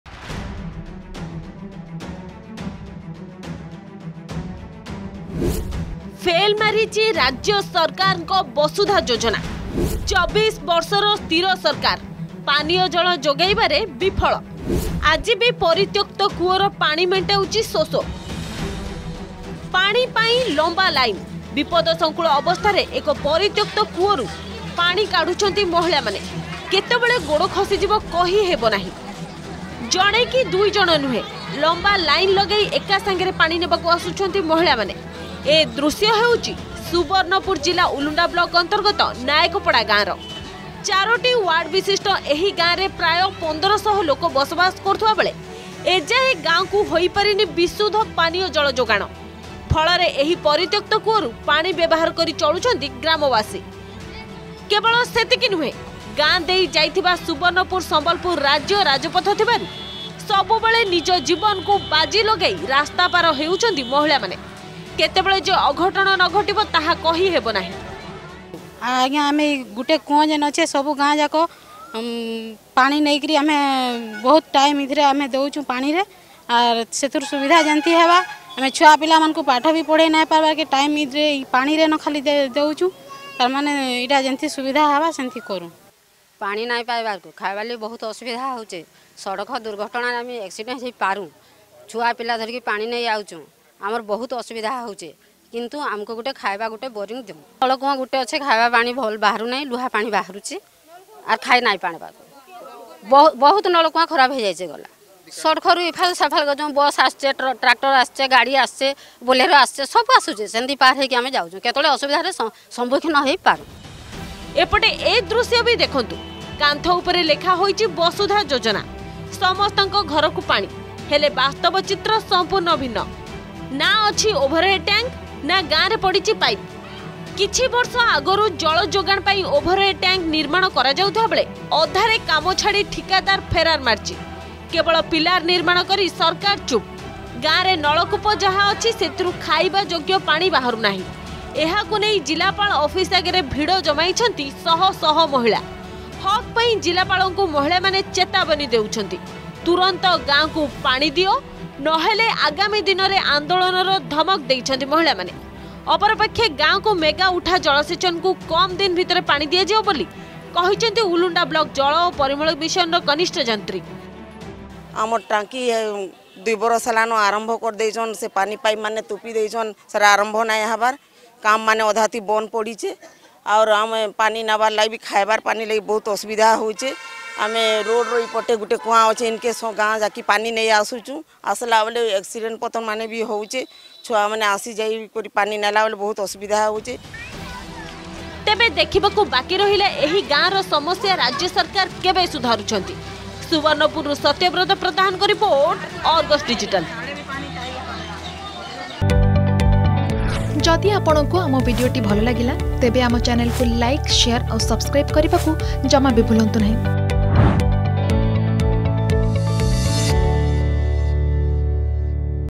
Water Crisis Deepens in Subarnapur: Locals Dependent on Well Water Amid BJD Govt Inaction in Odisha
ପାଣି ପାଇଁ ଲମ୍ବା ଲାଇନ । ବିପଦ ସଙ୍କୁଳ ଅବସ୍ଥାରେ ଏକ ପରିତ୍ୟକ୍ତ କୁଅରୁ ପାଣି କାଢୁଛନ୍ତି ମାହିଳାମାନେ ।